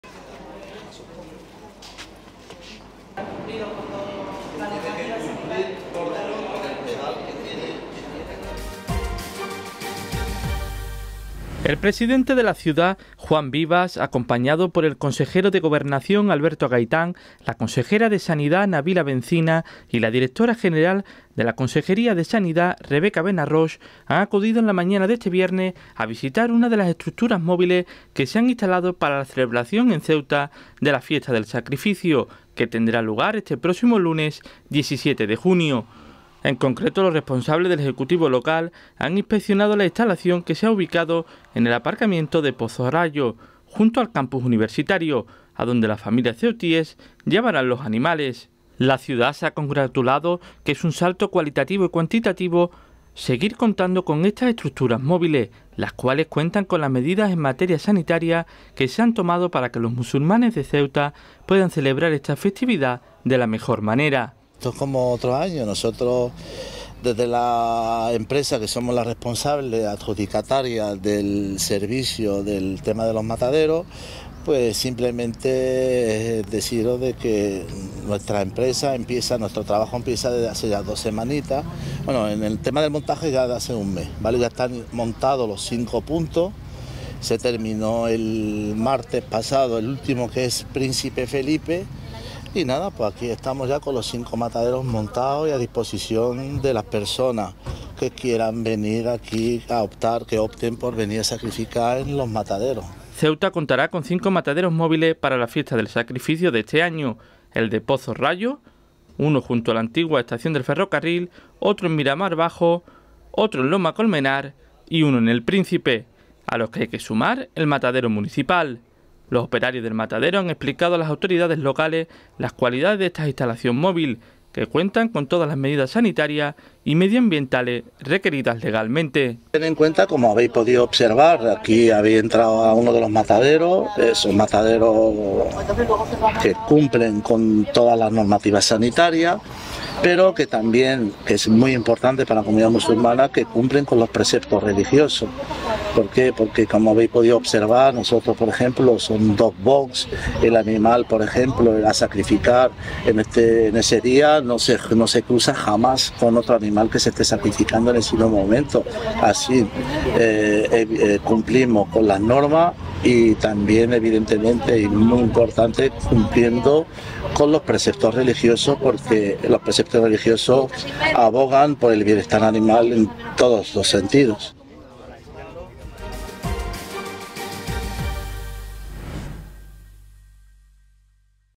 Los cinco mataderos móviles preparados para el Eid Al Adha
La Ciudad contará con estas instalaciones más el matadero público, todas ellas con las medidas sanitarias obligatorias para la Fiesta del Sacrificio.

Redacción y locución: Diego Naranjo
Imagen y edición: Joaquín Viera